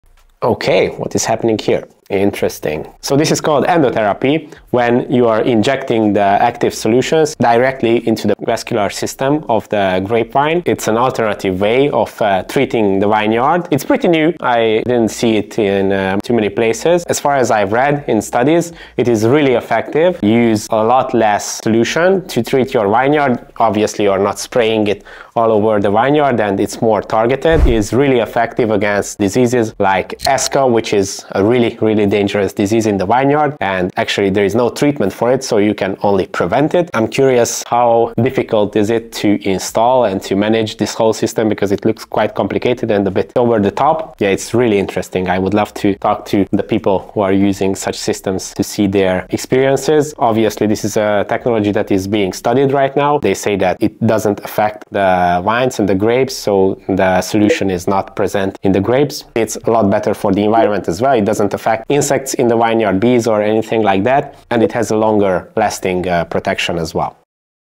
Vine Trunk Injections to Cure Disease?! 🍇💉🦠 (Endotherapy Explained)
Did you know vines can be treated like patients? 🍇💉🦠 In this video, we explore endotherapy — a cutting-edge technique where winemakers inject nutrients or treatments directly into the vine trunk to combat diseases such as Esca and Flavescence Dorée.
This precise, sustainable method reduces chemical use while protecting vine health from the inside out. Discover how science and viticulture meet to extend the life of vineyards around the world.
👉 If you love vineyard innovation and viticulture science, this one’s for you!